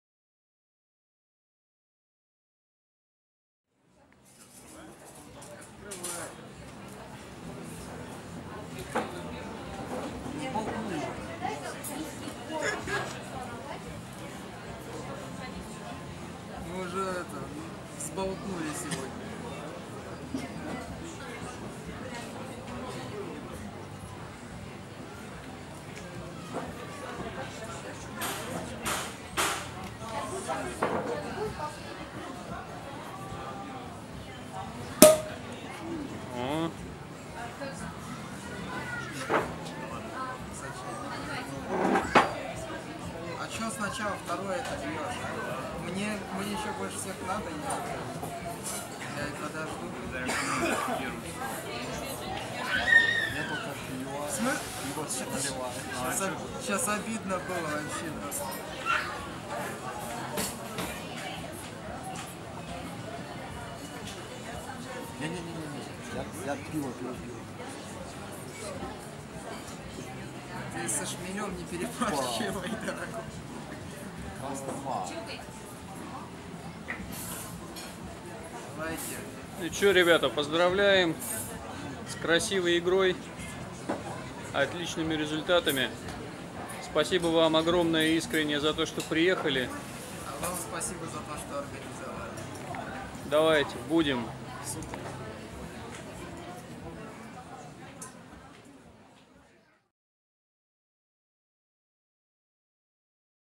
Игра точки | Шампанское на подиуме
Призеры Открытого Кубка Севастополя по старой спортивной традиции отмечают успешное выступление на турнире глотком игристого вина из только что завоеванного трофея.